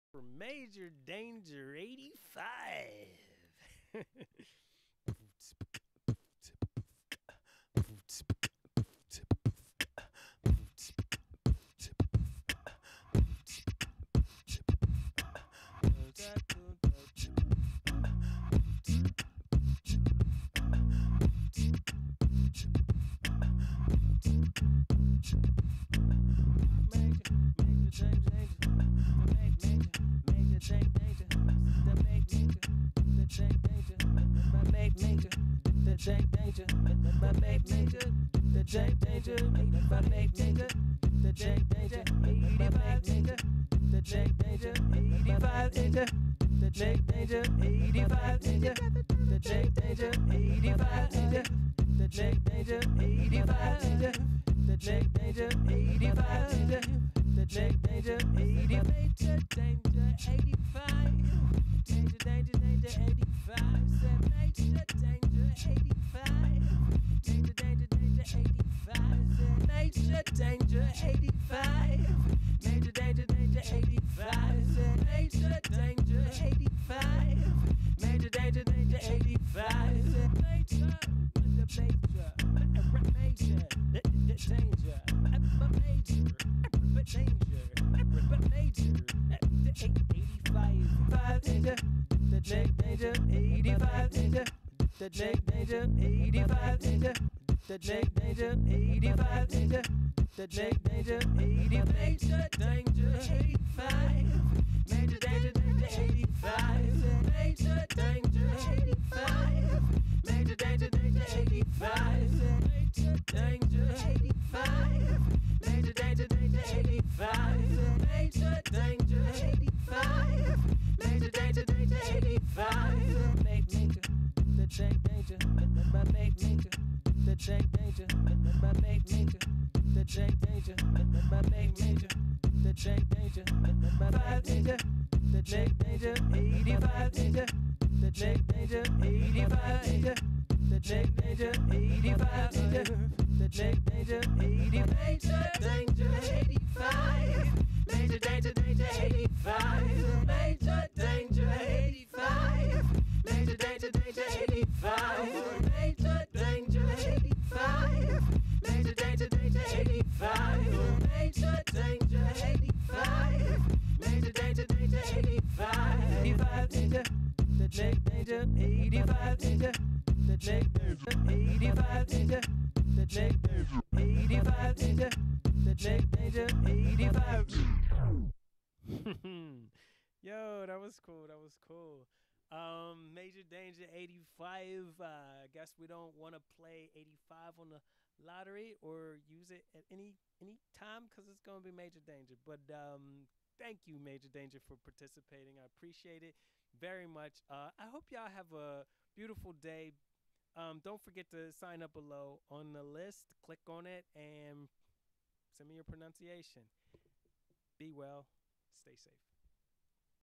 improv loops 4 u/MajorDanger85
Everyday I improv loop a user's name on Reddit, if you don't snoo you loose.